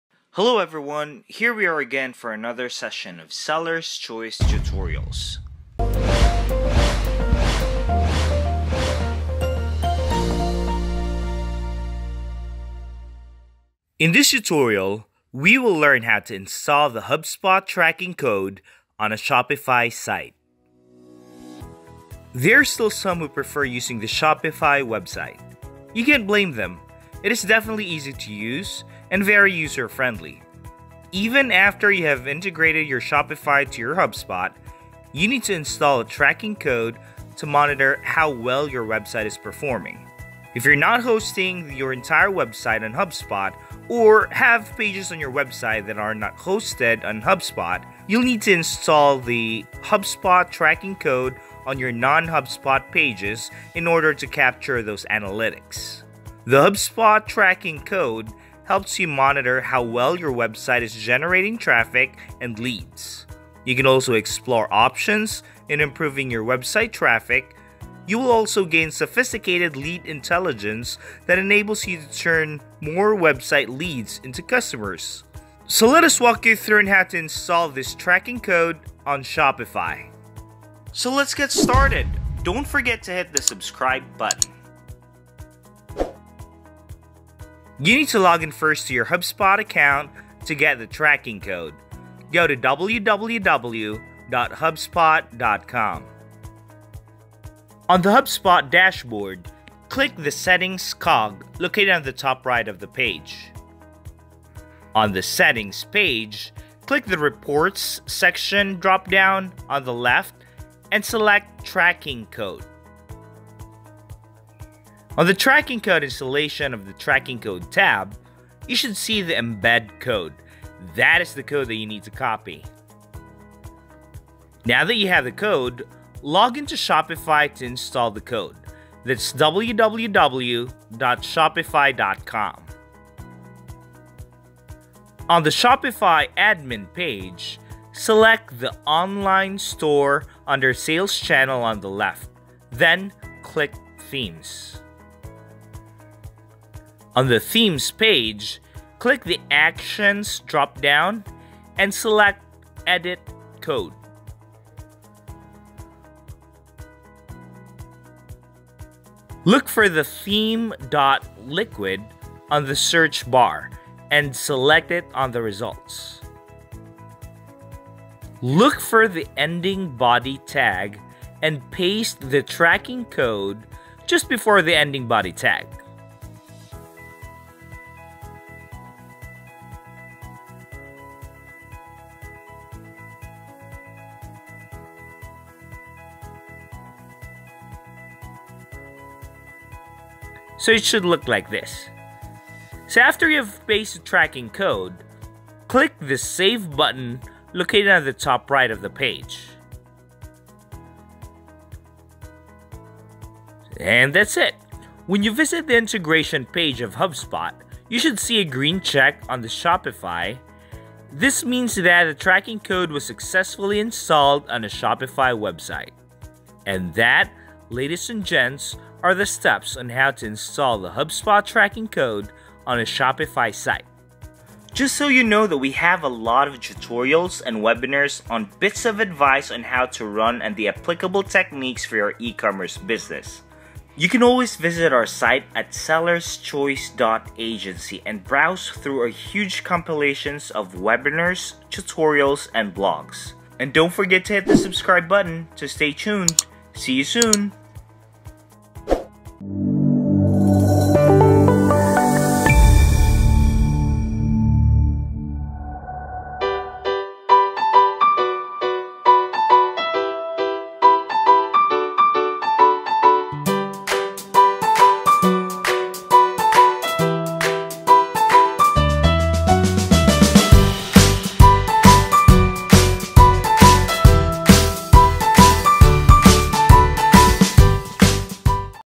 How to Install The HubSpot Tracking Code on a Shopify site
Today, we're showing you How to Install The HubSpot Tracking Code on a Shopify site.  Even after you have integrated your Shopify to your HubSpot, you need to install a tracking code to monitor how well your website is performing.